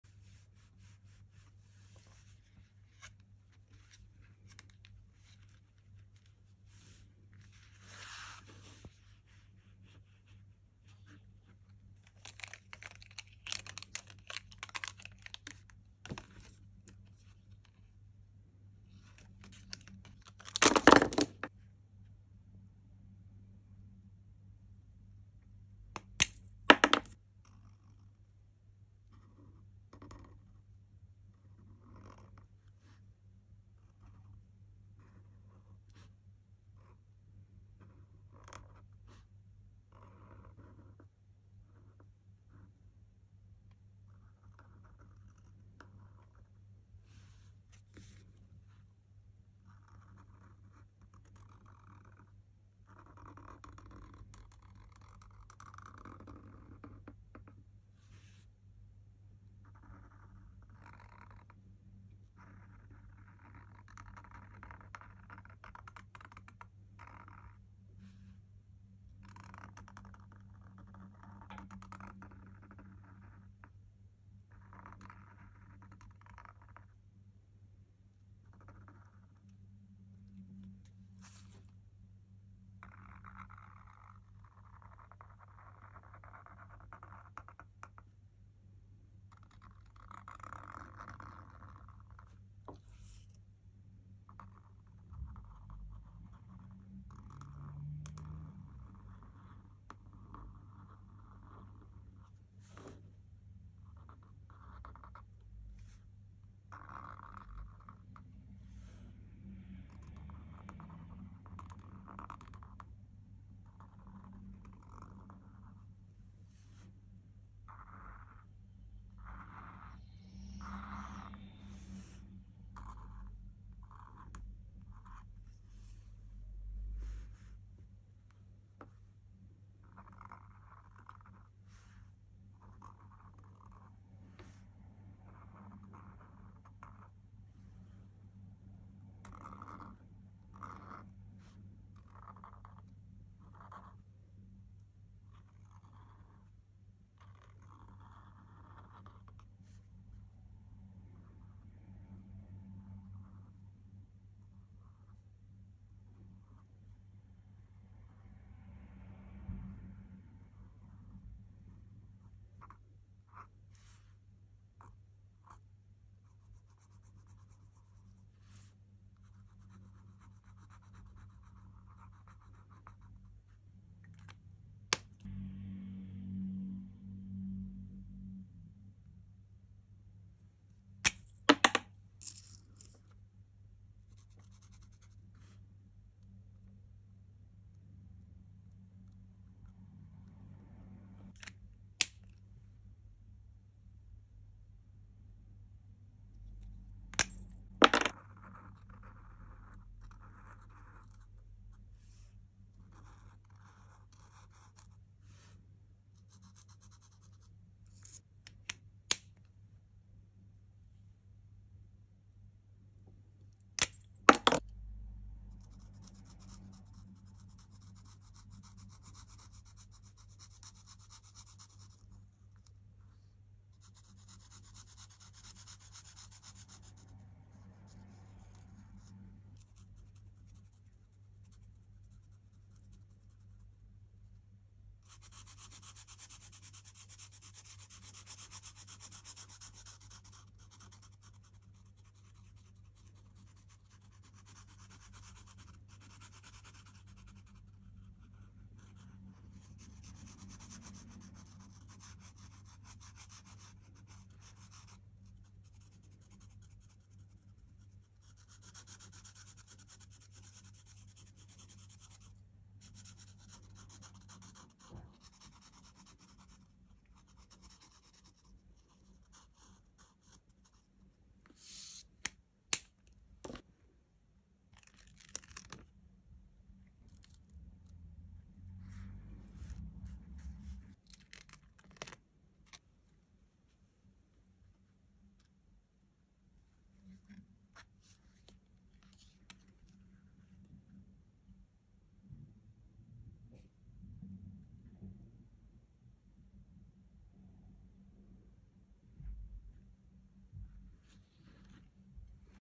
HOW TO DRAW HELLO KITTY VERY CUTE AND EASY🥺 DRAWING FOR BEGINNERS
Today I am drawing hello kitty very easy and simple steps to follow. I am drawing hello Kitty very cute Kawaii with ASMR in the background of pencils sounds and drawing sounds. Watch this quick and easy drawing tutorial to improve your artistic skills and you're one of the cutest Japanese hello kitty drawing. This drawing tutorial is for beginners drawing very easy and cute step-by-step. I have included ASMR to relax you and put you to sleep ASMR.

Markers: https
Markers 2
Camera: https: //amzn.to/3QkJ7yF
Lighting: https: //amzn.to/3WQFhzM
Editing Laptop
Table: https